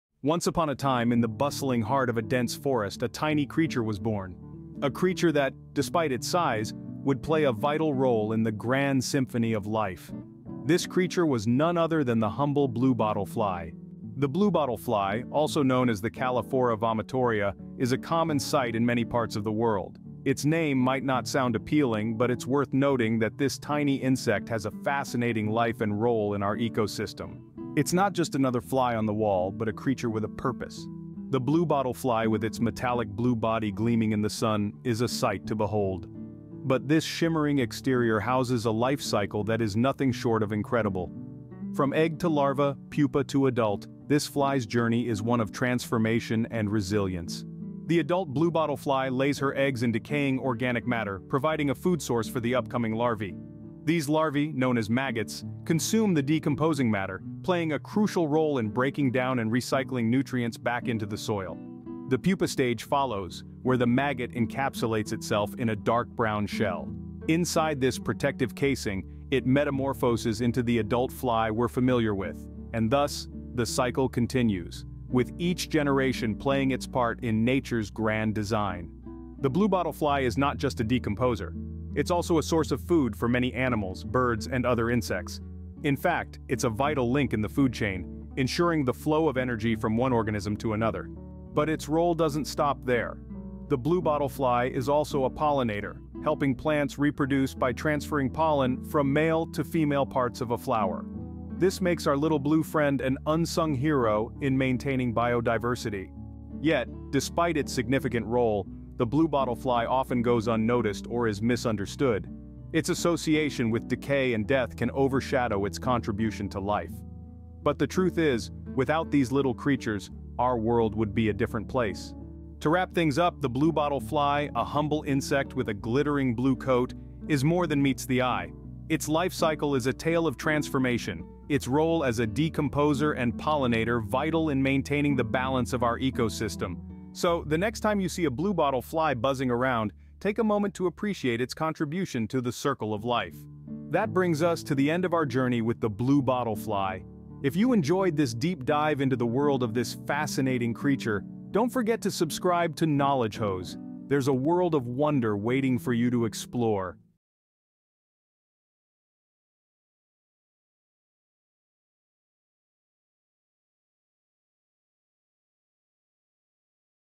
Bluebottle Fly - Decomposer And Pollinator
A YouTube video about Bluebottle Fly

The bluebottle fly, also known as Calliphora, is a species of fly that is distinguished by its distinctive coloration and loud buzzing flight. They are commonly found infesting carrion or excrement, and the larvae of some species may even kill sheep. Bluebottle flies can become a cause for concern when they appear in large numbers due to infested food matter or the carcass of a dead animal in which they have laid their eggs. The black blow fly is another widely distributed species with similar characteristics.